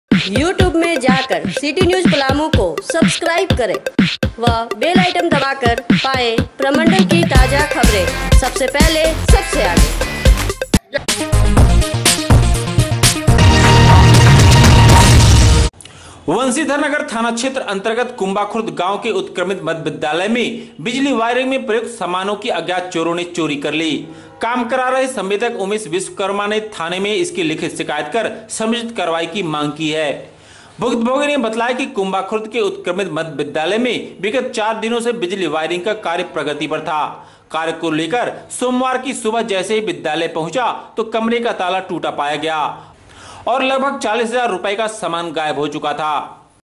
चोरो ने स्कूल से उड़ाये लगभग 40000 रुपये की मूल्य के बिजली सामान। मामला थाने में दर्ज
बंशीधर नगर थाना क्षेत्र के कुम्बा खुर्द गांव स्थित उत्क्रमिक मध्य विद्यालय में दिया चोरी की घटना को अंजाम